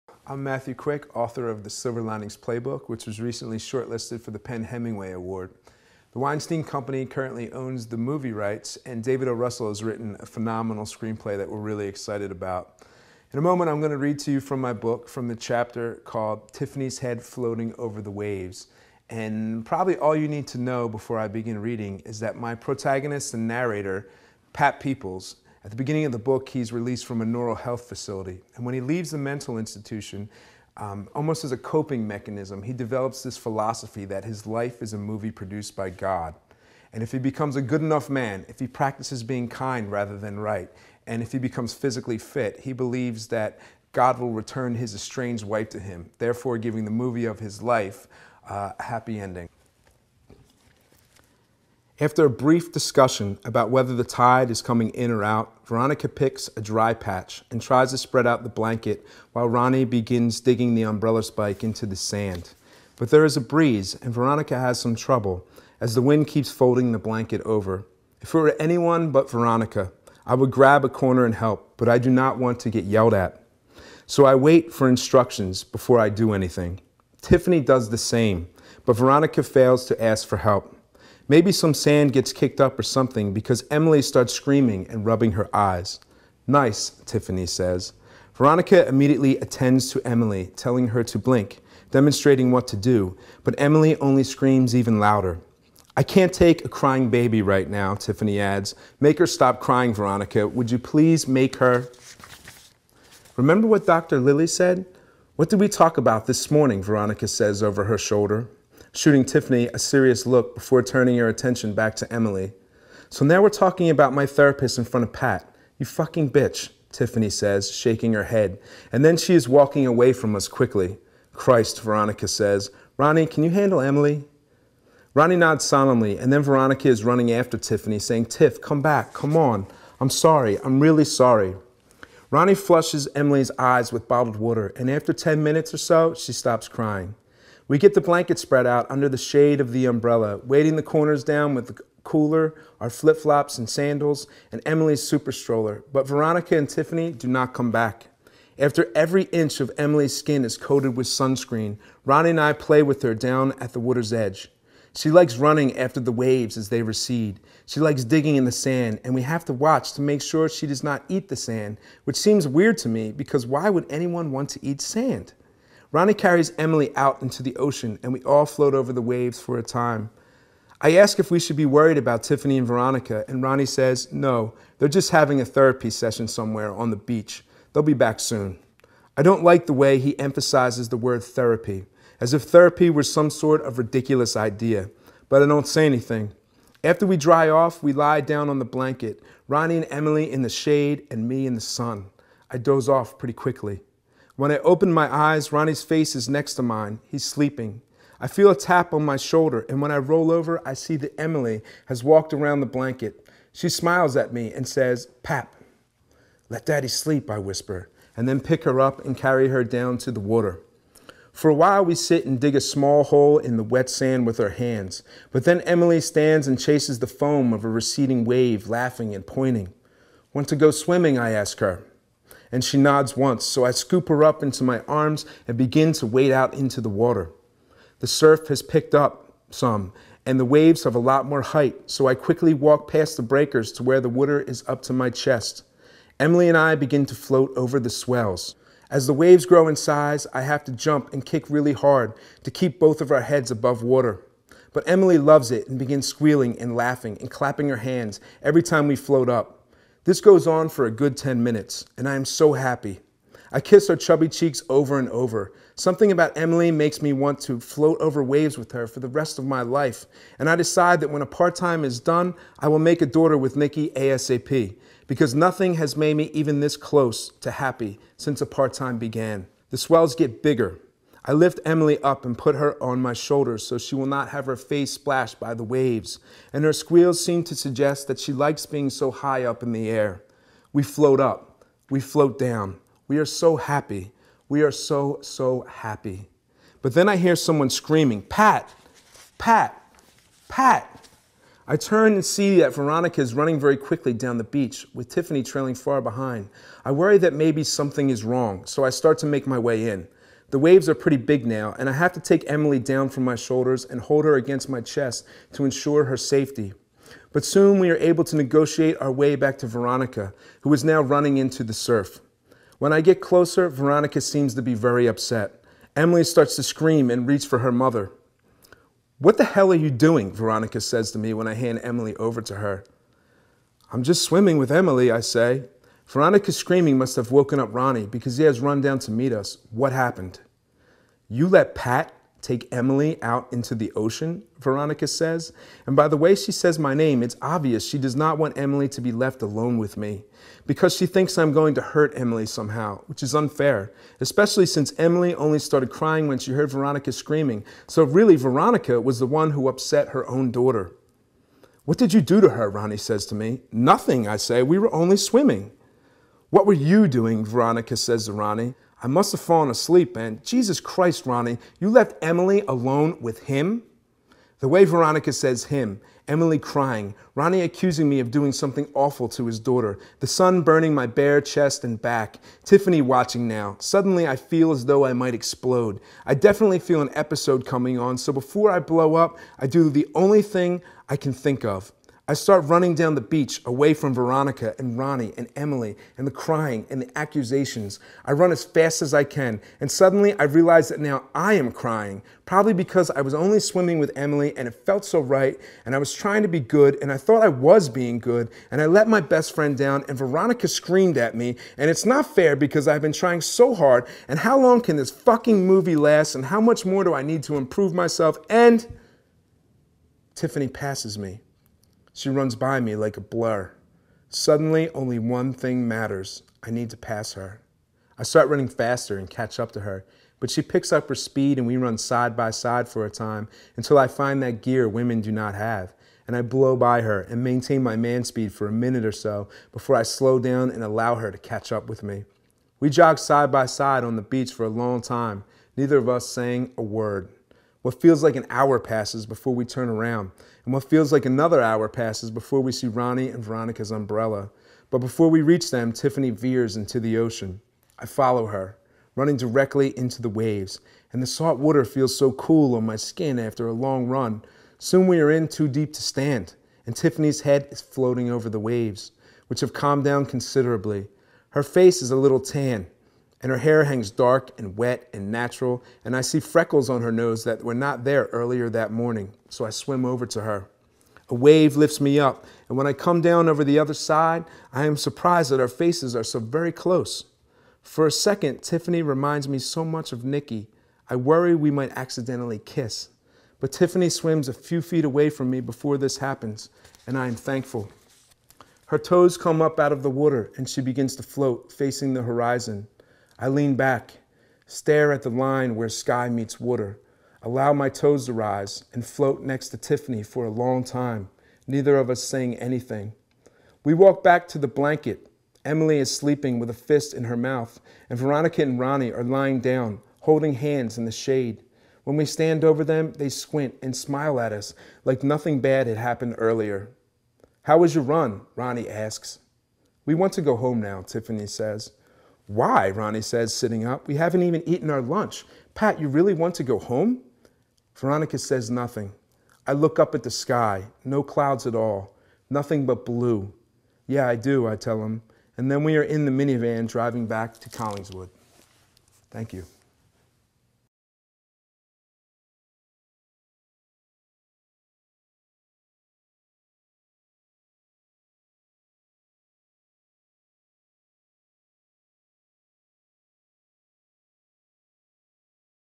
From THE SILVER LININGS PLAYBOOK, A Novel by Matthew Quick
Tiffany's Head Floating over the Waves: a chapter from THE SILVER LININGS PLAYBOOK: A Novel by Matthew Quick (Sarah Crichton Books / Farrar, Straus & Giroux 2008). The Weinstein Company has optioned the movie rights, and David O. Russell has written the screenplay. TSLP was short-listed for The PEN/Hemingway and lauded by National Public Radio, The Wall Street Journal, People, Kirkus Reviews, The Philadelphia Inquirer, Publishers Weekly, Pearl's Picks, and others. TSLP has been translated into Spanish and Italian and has been published in the UK by Picador.